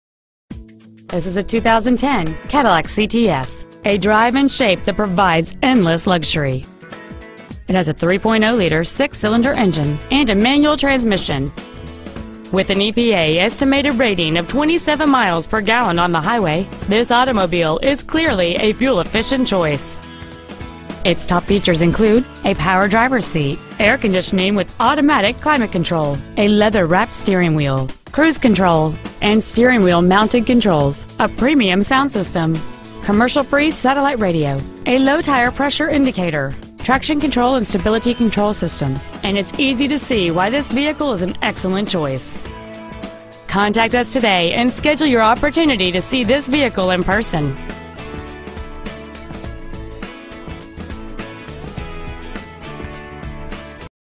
2010 Cadillac CTS Sedan Lincolnwood IL
We are proud to present this 2010 Cadillac CTS Sedan.

 Year: 2010
 Make: Cadillac
 Model: CTS Sedan
 Engine: 3.0L V6
 Trans.: Manual
 Exterior: Black

 Interior: Black

 6900 N. McCormick Blvd